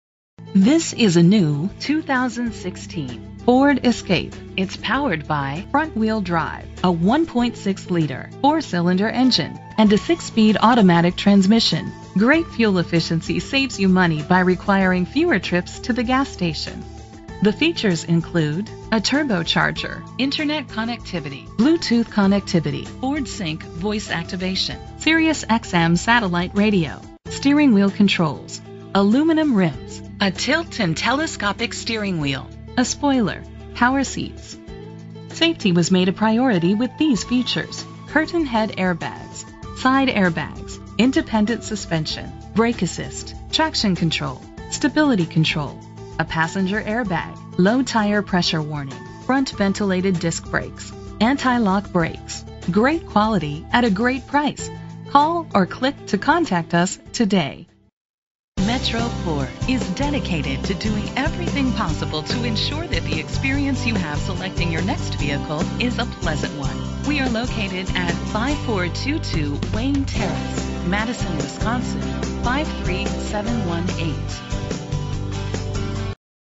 2016 Ford Escape - Madison WI
Phone:
Year: 2016
Make: Ford
Model: Escape
Trim: SE
Engine: 1.6 liter inline 4 cylinder 16 valve
Transmission: 6-Speed Automatic
Color: Oxford White
Mileage: 376
Address:
5422 Wayne Terrace